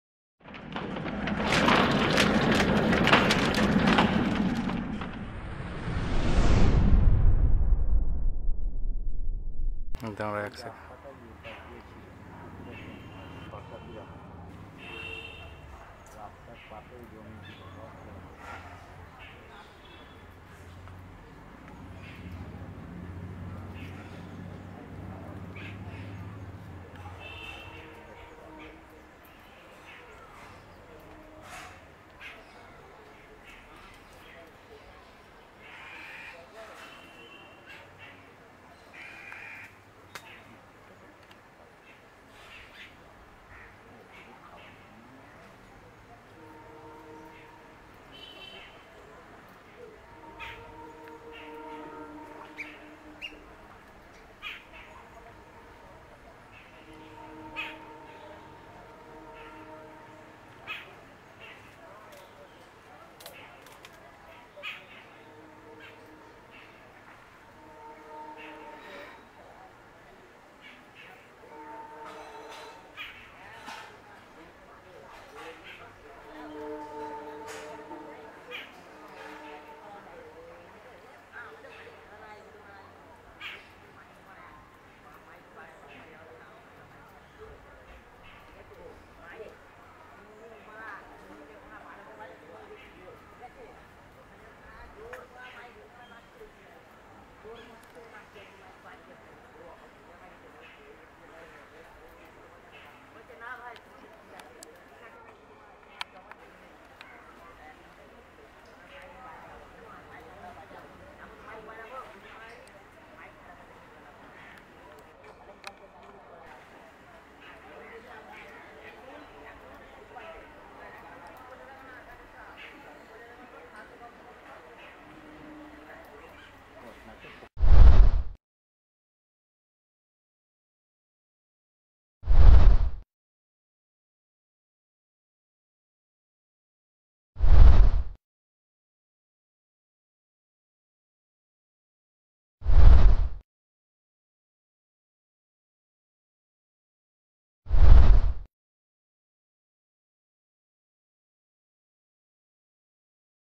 Squirrels Eating Food at Behind our Home | Art Villa
Squirrels are members of the family Sciuridae, a family that includes small or medium-size rodents. The squirrel family includes tree squirrels, ground squirrels, chipmunks, marmots, flying squirrels, and prairie dogs amongst other rodents.
Scientific name: Sciuridae
Family: Sciuridae; Fischer de Waldheim, 1817
Class: Mammalia
Lifespan: Siberian chipmunk: 6 – 10 years, Alpine marmot: 15 – 18 years
Gestation period: Eastern gray squirrel: 44 days, Siberian chipmunk: 28 – 35 days, Alpine marmot: 33 – 34 days
Did you know: It takes about three weeks before a baby squirrel will even open its eyes. cottagelife.com.